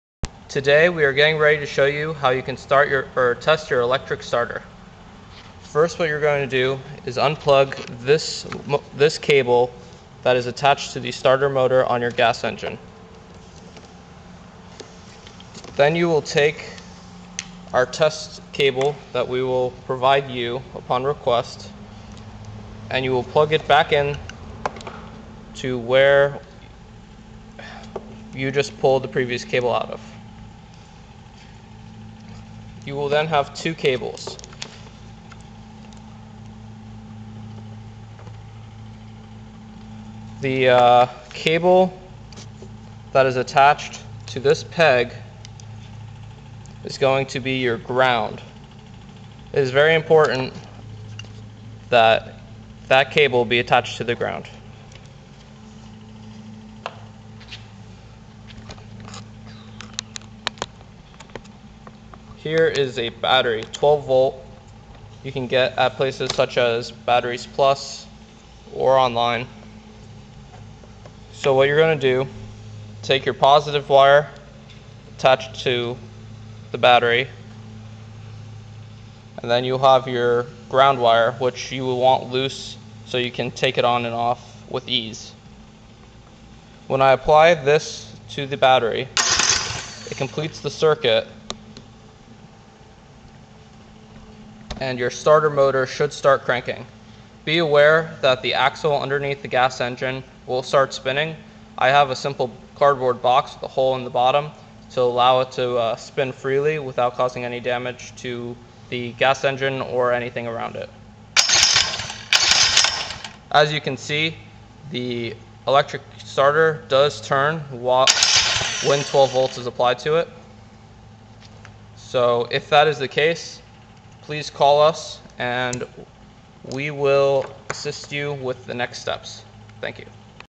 HOW TO TEST THE ELECTRIC START
How to test the electric start on a small gas engine.